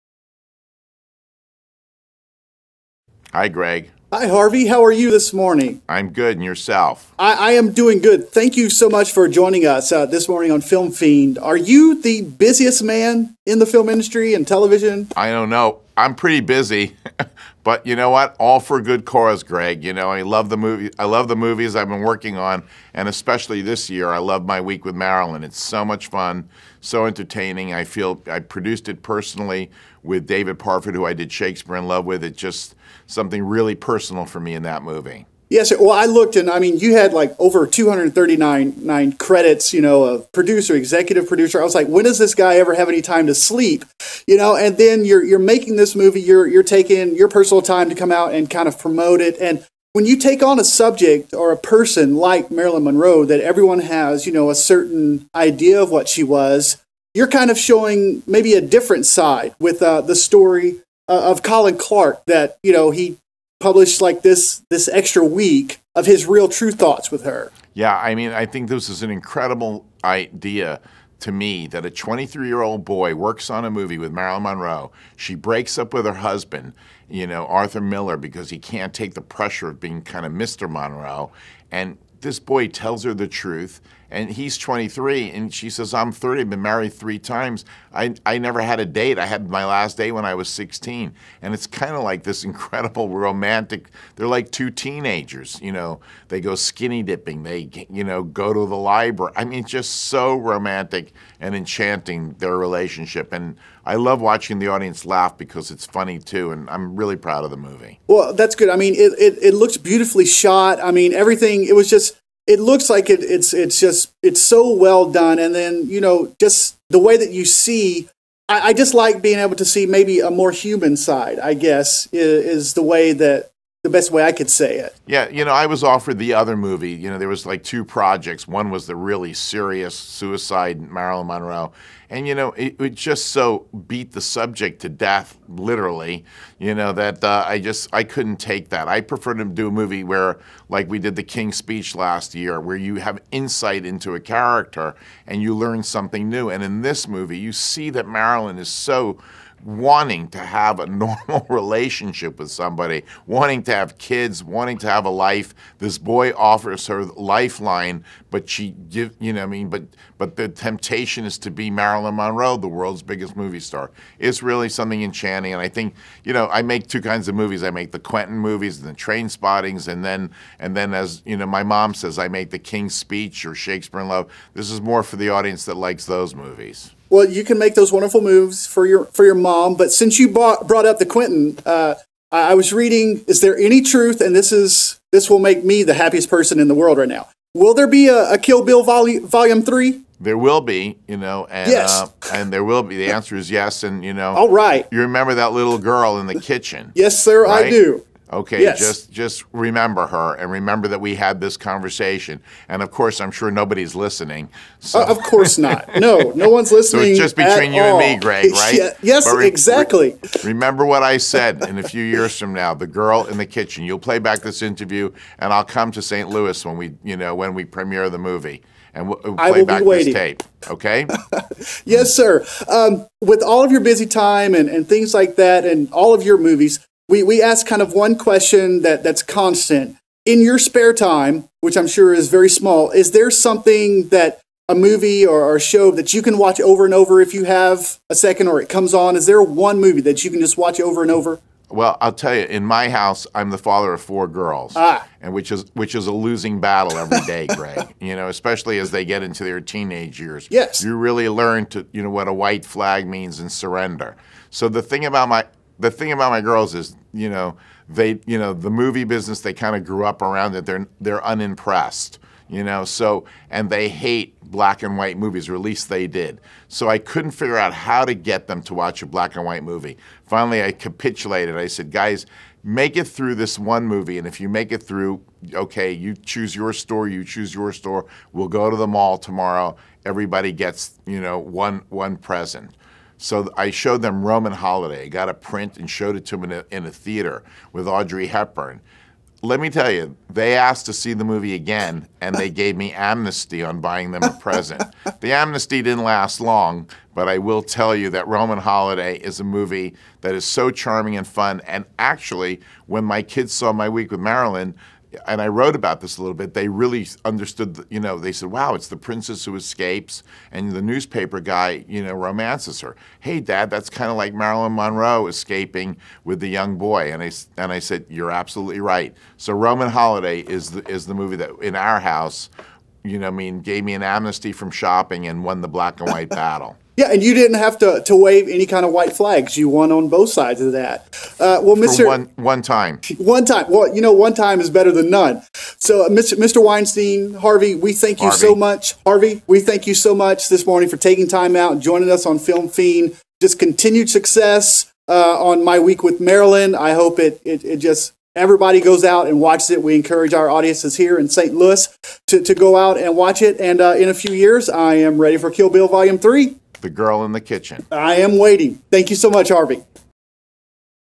Film Fiend | Harvey Weinstein | My Week With Marilyn
Harvey spills the beans. Remember the little girl in the kitchen scene in Kill Bill 2?